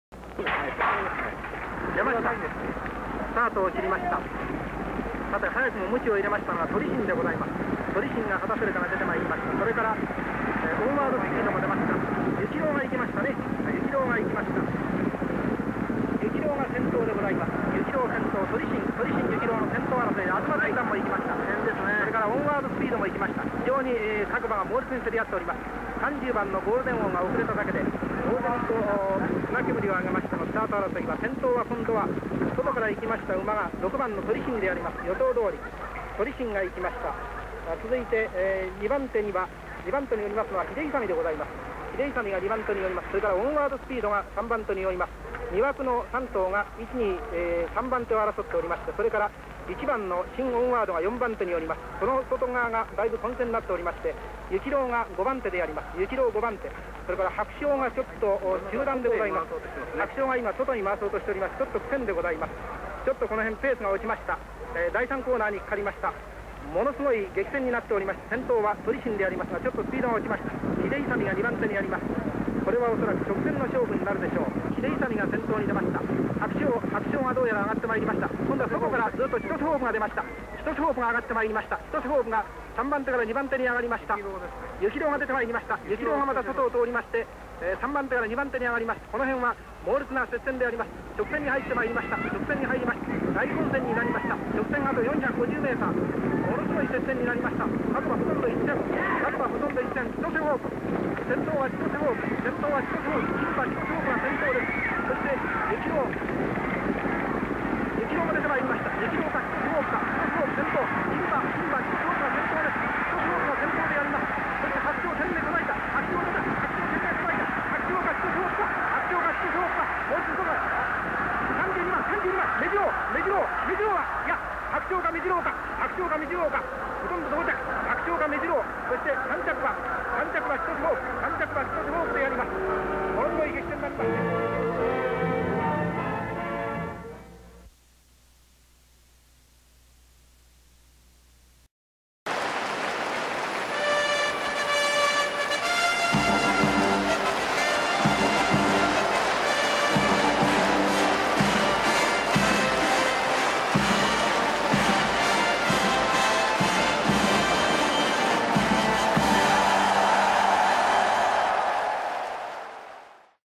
第２８回 東京優駿（日本ダービー） ハクシヨウ 牡３ 57 保田隆芳 1961.5.28 出走馬：メジロオー、チトセホープ、ケンカツプ、ユキロウ 、カネアサヒ、キンテキ、リユウライト、アサカブト他
第２８回　東京優駿（日本ダービー）
1961/5/28　東京9R　3歳牡牝　定量　芝2400m（左）　晴　良
本賞金（万円）　1着700/2着280/3着140/4着70/5着55
付加賞（万円）　1着128.52/2着36.72/3着18.36
※年齢表記は満年齢（新表記）に統一する。
1着 3枠 13番 ハクシヨウ 牡３ 57 保田隆芳 尾形藤吉 R2:30.2 1人
2着 6枠 32番 メジロオー 牡３ 57 八木沢勝美 大久保末吉 ハナ 23人
3着 5枠 24番 チトセホープ 牝３ 55 伊藤修司 伊藤勝吉 １身 2人
4着 2枠 7番 ケンカツプ 牡３ 57 藤本勝彦 藤本冨良 １ 1/2身 8人
5着 4枠 17番 ユキロウ 牡３ 57 渡辺正人 松山吉三郎 3/4身 5人
6着 1枠 4番 カネアサヒ 牡３ 57 新関力 杉浦照 １身 15人
7着 5枠 22番 キンテキ 牡３ 57 森安重勝 古賀嘉蔵 3/4身 24人
8着 6枠 27番 リユウライト 牡３ 57 宮本悳 橋本正晴 ２身 6人
9着 3枠 15番 アサカブト 牡３ 57 増沢末夫 佐々木猛 クビ 17人
10着 4枠 20番 グランドタイム 牡３ 57 境勝太郎 中野吉太郎 ２身 10人
11着 5枠 26番 キヨクコウ 牡３ 57 梶与四松 二本柳俊夫 1/2身 16人
12着 5枠 25番 ライジングスター 牡３ 57 山岡忞 中村広 ２身 18人
13着 2枠 9番 チトセミドリ 牡３ 57 松永高徳 伊藤勝吉 クビ 12人
14着 4枠 16番 イカホ 牡３ 57 加賀武見 奥平作太郎 1/2身 3人
15着 1枠 3番 ケンロクオー 牡３ 57 古山良司 藤本冨良 1/2身 3人
16着 5枠 21番 マツノハナ 牡３ 57 石毛善衛 田中朋次郎 1/2身 32人
17着 5枠 23番 スズカリユウ 牡３ 57 中尾銑治 橋本正晴 ２身 28人
18着 3枠 14番 アズマテンラン 牡３ 57 高橋英夫 二本柳俊夫 3/4身 4人
19着 6枠 31番 ハードウイン 牡３ 57 野平祐二 松山吉三郎 3/4身 14人
20着 2枠 8番 ヒデイサミ 牡３ 57 池之上豊 松永光雄 アタマ 7人
21着 6枠 29番 スズキオー 牡３ 57 飯塚好次 松山吉三郎 １ 1/2身 31人
22着 3枠 11番 フジフレーム 牡３ 57 伊藤竹男 久保田金造 クビ 9人
23着 2枠 10番 オンワードスピード 牡３ 57 坂本栄三郎 二本柳俊夫 3/4身 20人
24着 1枠 5番 ハルノオー 牡３ 57 小野定夫 橋本輝雄 クビ 19人
25着 1枠 2番 トウレイホープ 牡３ 57 内田守 見上恒芳 アタマ 27人
26着 6枠 30番 ゴールデンオー 牡３ 57 宇賀神英治 浅野武志 アタマ 29人
27着 4枠 19番 シユマーレンゼー 牡３ 57 森安弘明 見上恒芳 アタマ 30人
28着 2枠 6番 トリシン 牡３ 57 丸目敏栄 荒木静雄 ２身 21人
29着 6枠 28番 カズサ 牡３ 57 高松三太 田中朋次郎 ６身 22人
30着 3枠 12番 ヤマミノル 牡３ 57 浅見国一 藤本冨良 １身 26人
31着 4枠 18番 ヨドノハル 牡３ 57 野平好男 伊藤勝吉 ７身 25人
32着 1枠 1番 シンオンワード 牡３ 57 田村駿仁 二本柳俊夫 １ 1/4身 11人

馬名 ハクシヨウ Ⅱ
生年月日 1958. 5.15
生産地 北海道浦河
生産者 酒井牧場
性別 牡
毛色 黒鹿毛
調教師 尾形藤吉（東京）
馬主 西博
競走成績 ９戦７勝
受賞歴 1960年　啓衆者賞最優秀３歳牡馬
獲得賞金 12,602,850円